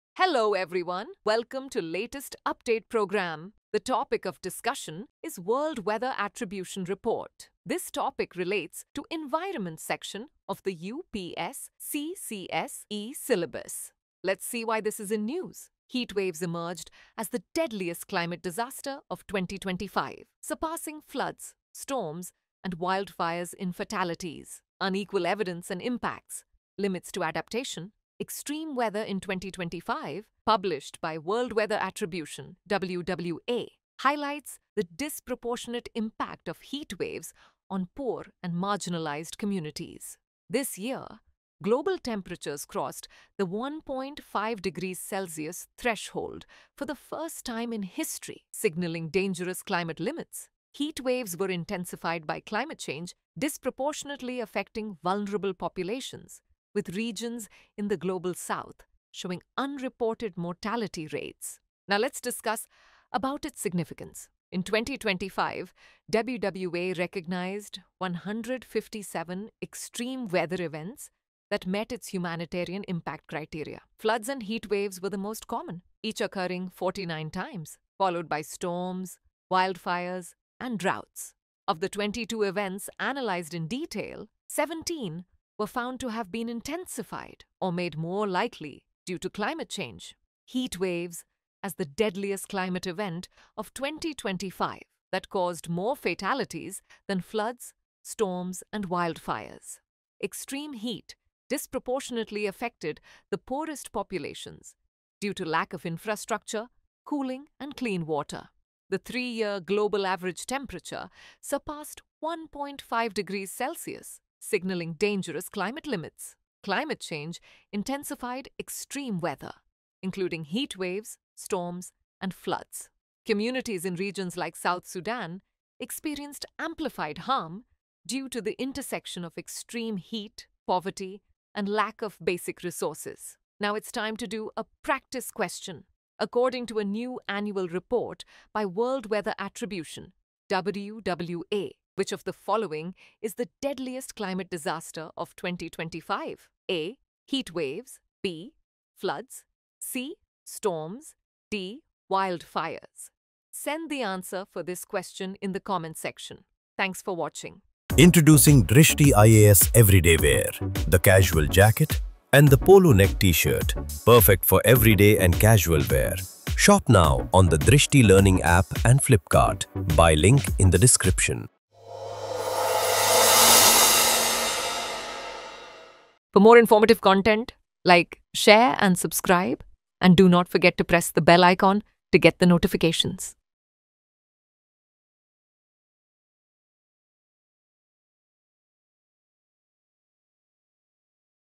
2025 World Weather Attribution Report | Key Findings | Latest Update | Drishti IAS English
The 2025 World Weather Attribution (WWA) report identifies heatwaves as the deadliest climate disaster of the year, surpassing floods, storms, and wildfires in fatalities. Climate change has intensified extreme weather, disproportionately affecting poor and marginalized communities. For the first time, global temperatures have crossed the 1.5°C threshold, signalling dangerous climate limits. While adaptation measures can mitigate impacts, the report warns that climate change is pushing societies toward the limits of adaptation, particularly in low-income regions.

Dear Viewers,